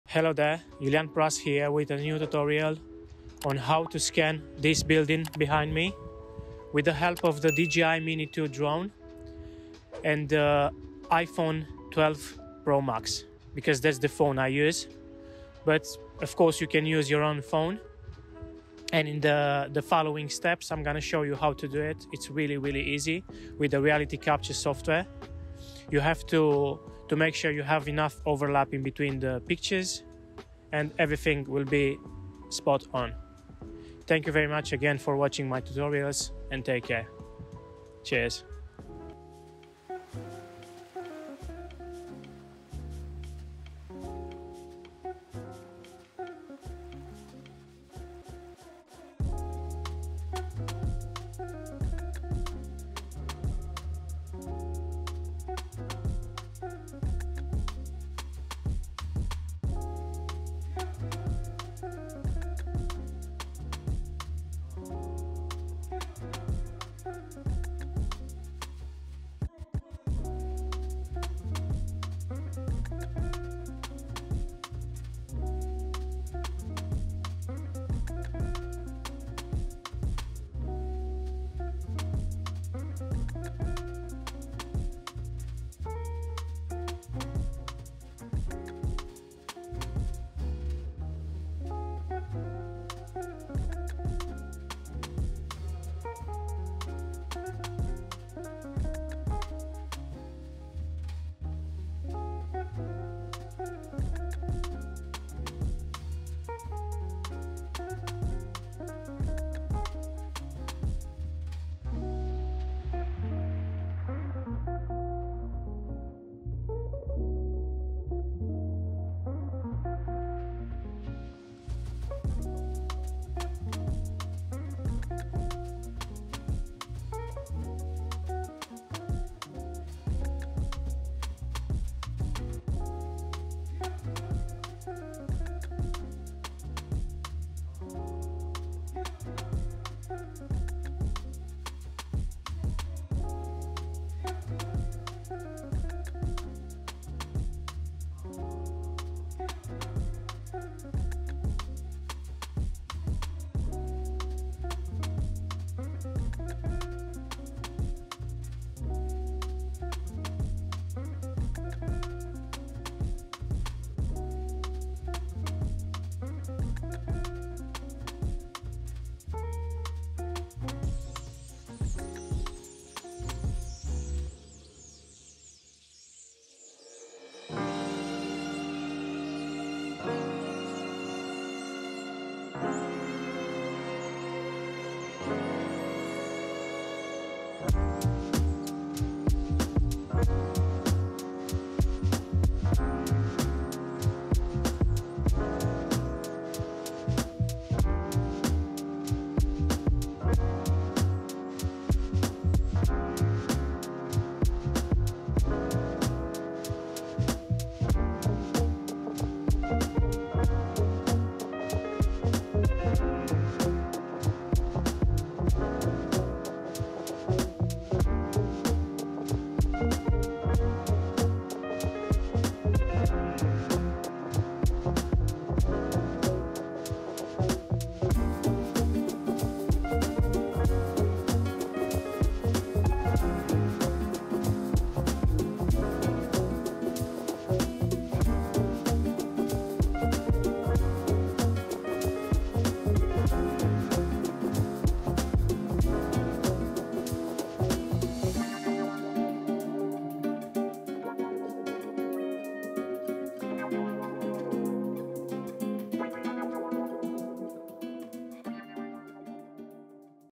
UNBELIEVABLE 3D Scanning for FREEE using any phone
In today's tutorial I will show you how a scan a building with a phone and a drone.